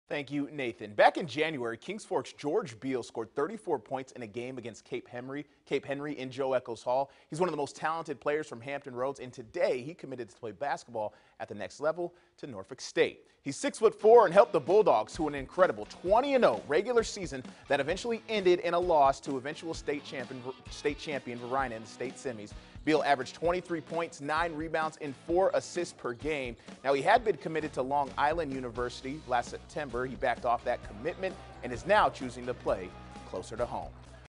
George Beale picks Norfolk State
Beale averaged 23 ppg in his senior season at King's Fork in Suffolk, VA